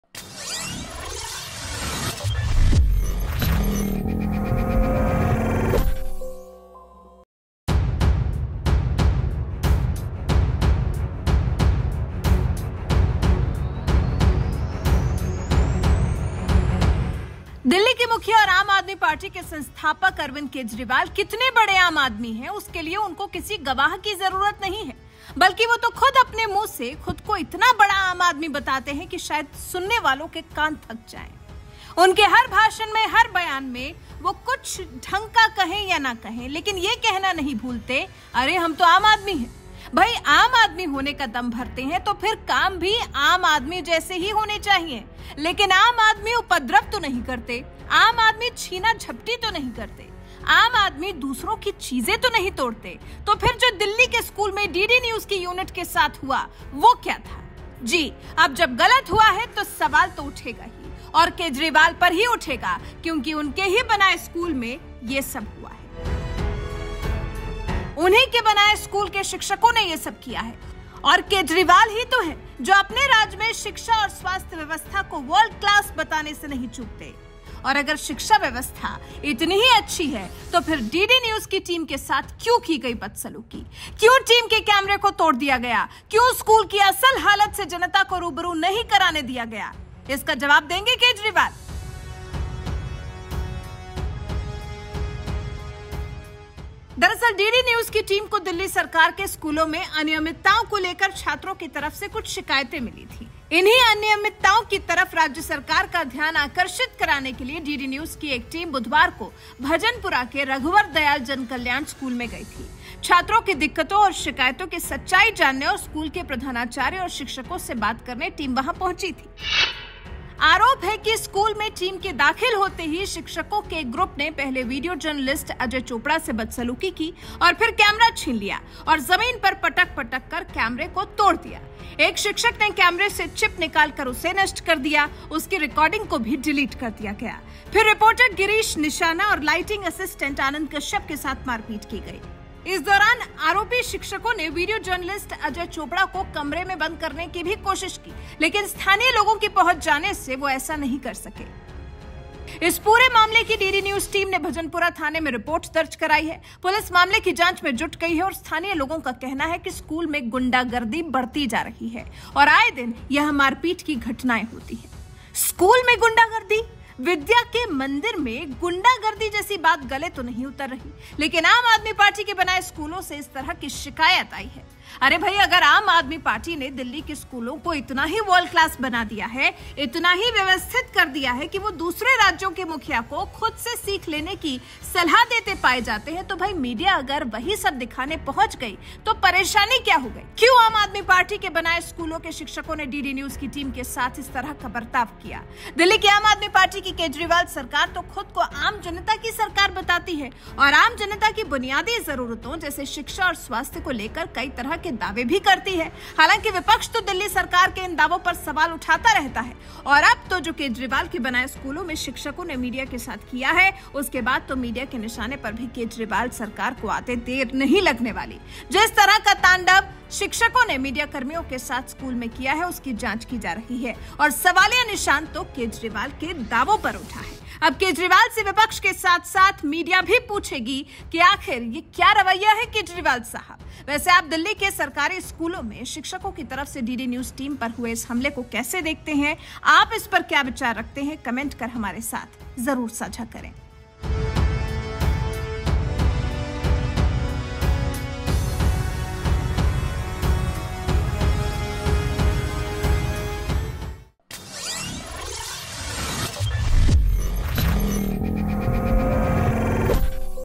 Kejriwal के स्कूल का हाल जानने पहुंची DD News की टीम को शिक्षकों ने पीटा, कैमरा भी तोड़ दिया !
दिल्ली में केजरीवाल के बनाए स्कूल की मिल रही शिकायतों पर हाल जानने जब मीडिया की टीम पहुँची तो शिक्षकों ने कैमरा तोड़कर पत्रकारों के साथ मारपीट कर दी। इसपर क्या कहेंगे आम आदमी केजरीवाल?

Producer Nivedita
Editor Neha